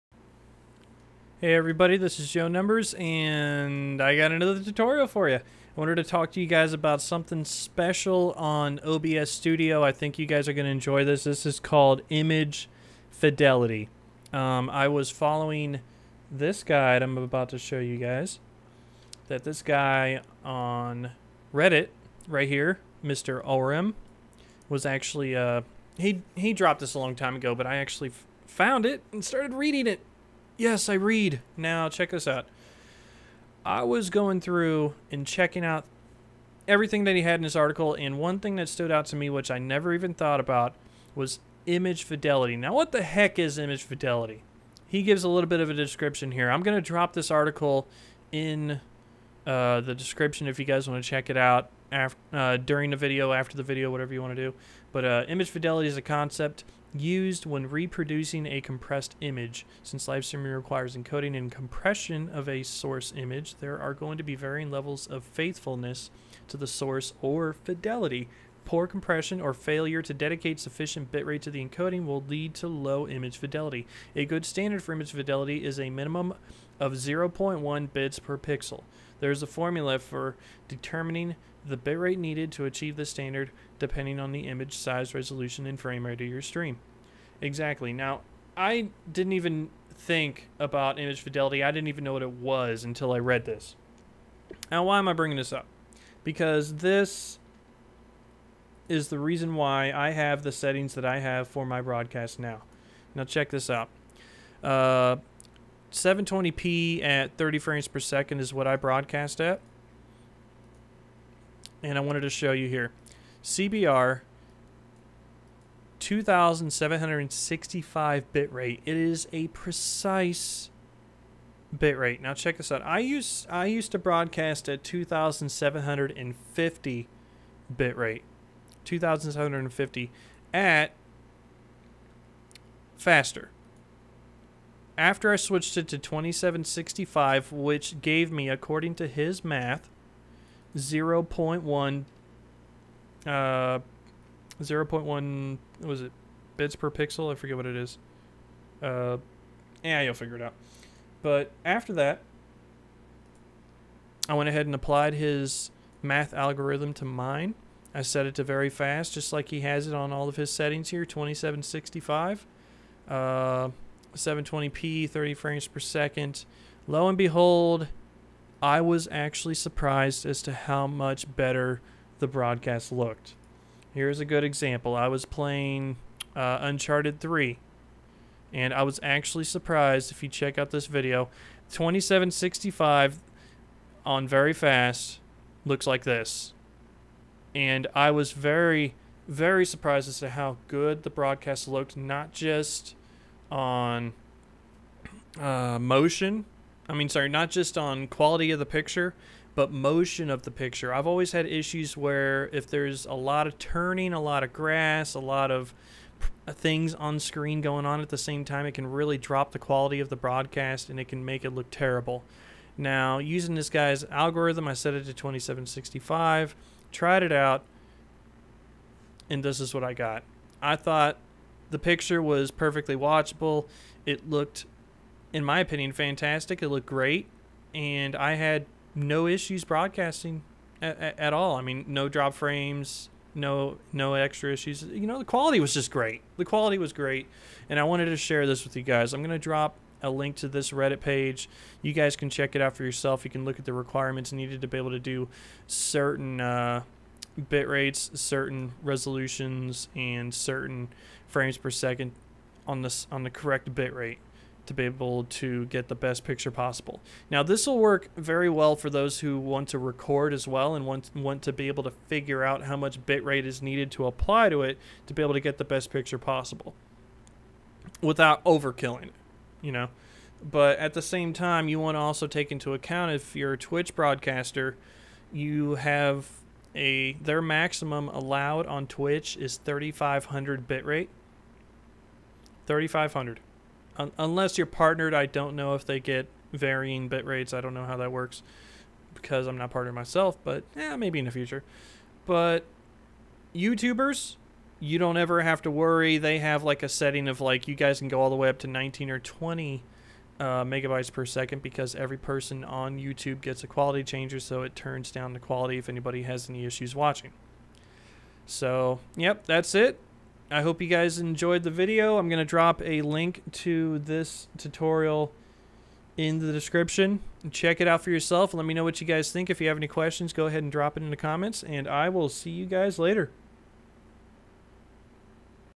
OBS Studio Tutorial - 0.14.2 - Image Fidelity - Best Bitrate for Quality!
I tried this and I love the quality. Hope it works out for you as well :D